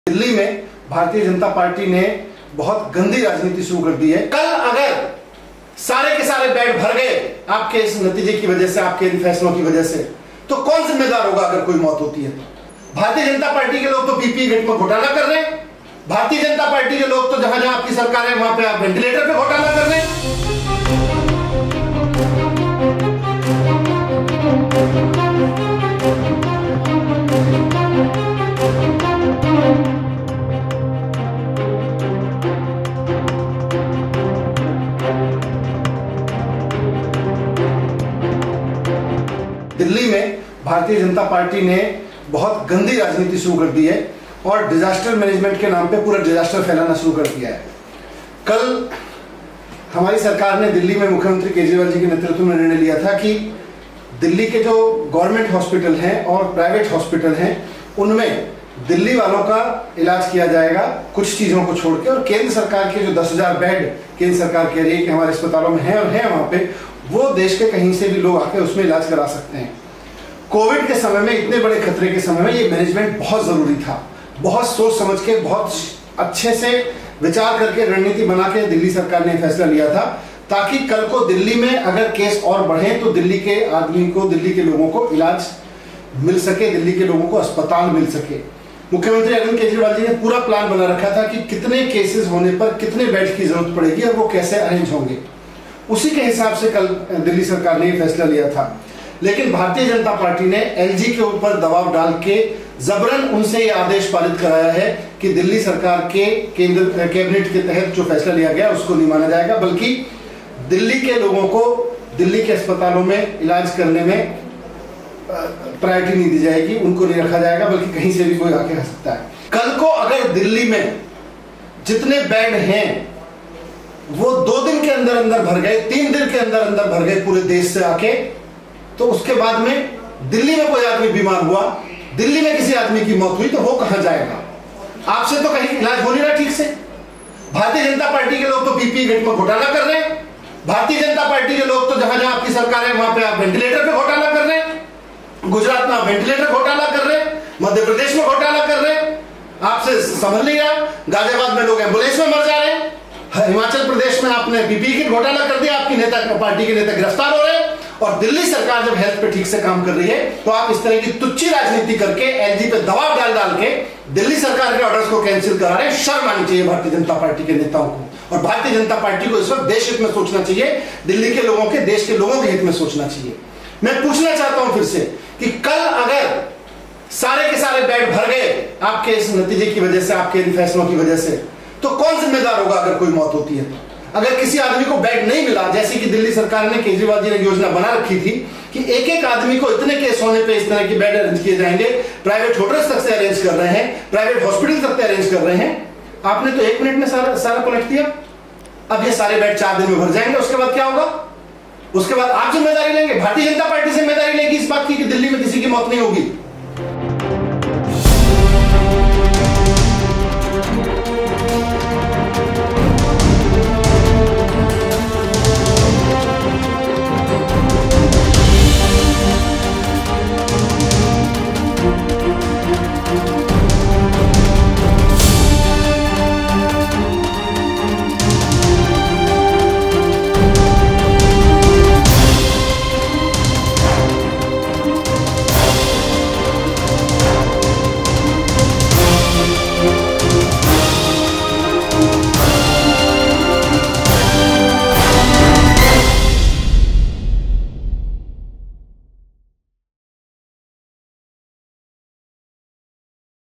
‘Dirty politics’: Sisodia lashes out at BJP as AAP’s decision on beds overruled
Delhi’s Deputy Chief Minister Manish Sisodia lashed out at BJP after Delhi’s L-G overruled AAP government’s decision of reserving beds in hospitals. Sisodia accused BJP of playing dirty politics during the time of pandemic. “Centre should be working with State government but instead BJP is playing dirty politics,” Sisodia said. Delhi government had earlier announced that hospitals under state government will be reserved for people residing in Delhi.